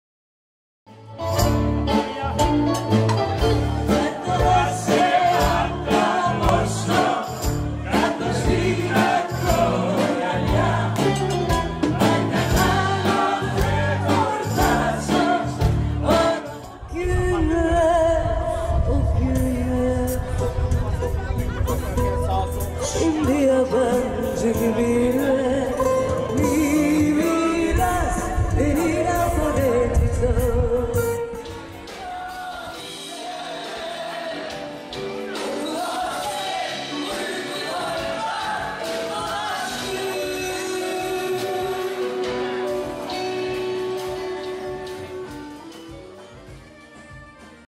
ARZU ÖZCANOĞLU TANITIM 2019
Vega Ajans ailesinin değerli üyesi; Ses sanatçısı Arzu Özcanoğlu; kurumsal markaların özel gecelerinde Türk ve Yunan müziğinden oluşan seçkin repertuvarı ile sahne almaktadır.